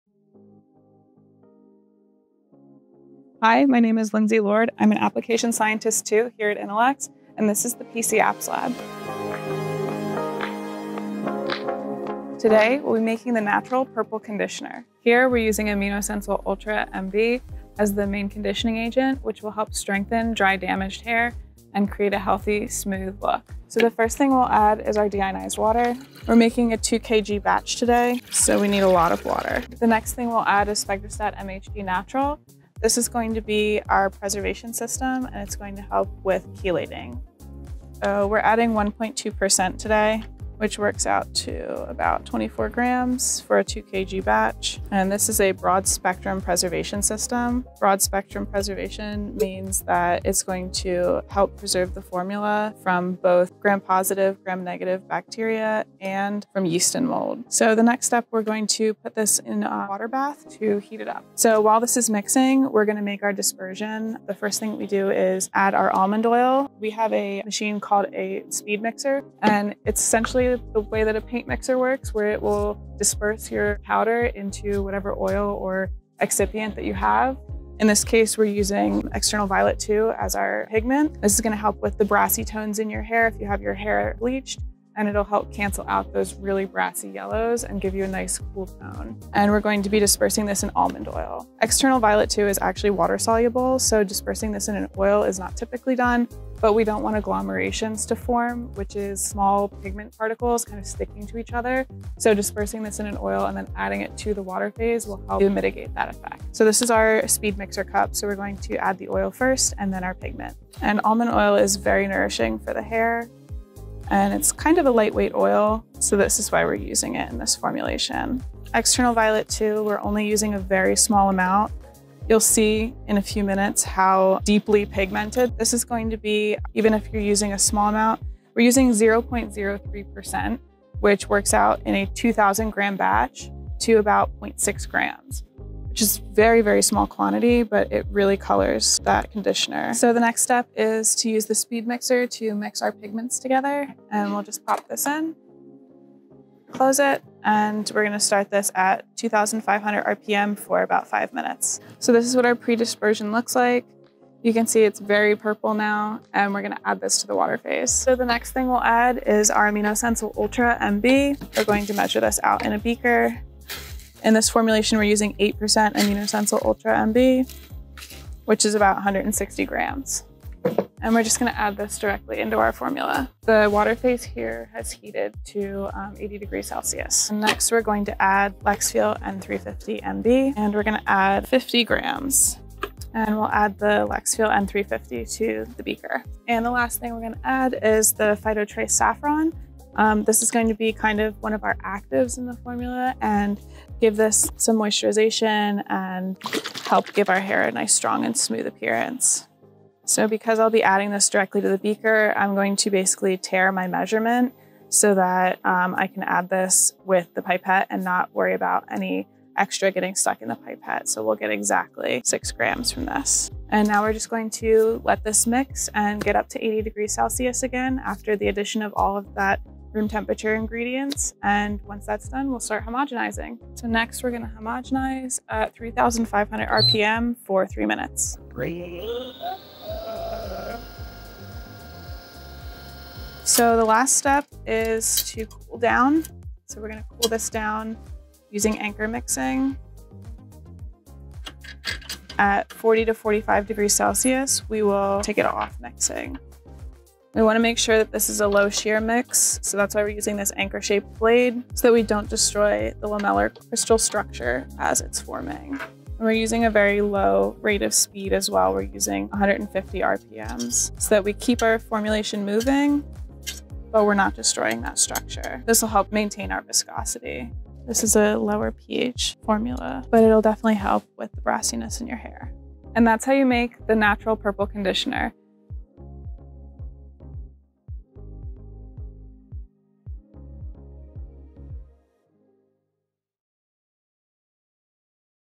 Formulating with AminoSensyl™ Ultra MB
In the lab with Lindsay Lord, Application Scientist II, as she takes us through the formulation steps to create a Natural Purple Conditioner prototype using AminoSensyl™ Ultra MB, a breakthrough conditioning for sustainable BTAC replacement. This ~100% natural formulation inspiration keeps hair bright and healthy, powered by AminoSensyl™ Ultra MB amino lipid technology, a non-quat complete conditioning system with cationic amino lipid technology.